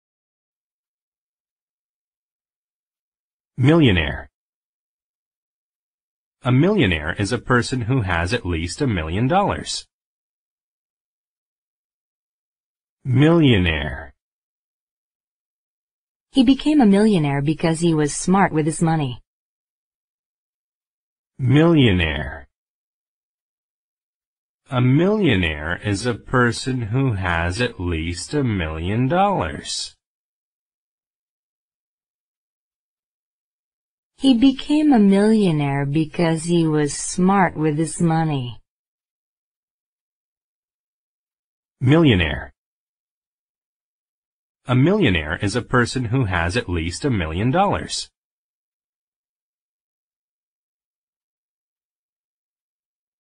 Millionaire : B1 level english vocabulary lesson
In this english video we will see what the word "Millionaire" means. It's an English word from B1 level vocabulary list.

The word is "Millionaire", meaning is: "A millionaire is a person who has at least a million dollars." and example is: "He became a millionaire because he was smart with his money.".